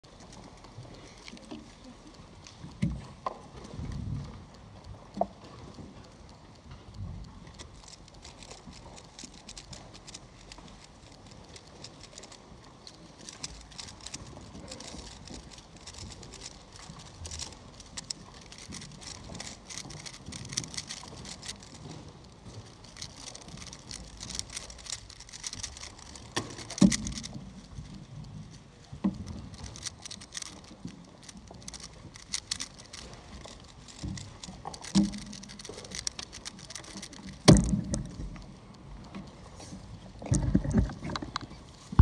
32.90 Official 3BLD Single![World Championship 2019]
Second fastest officially! Dnfed solve 2 because of a timer reset, but it was a 50 anyway.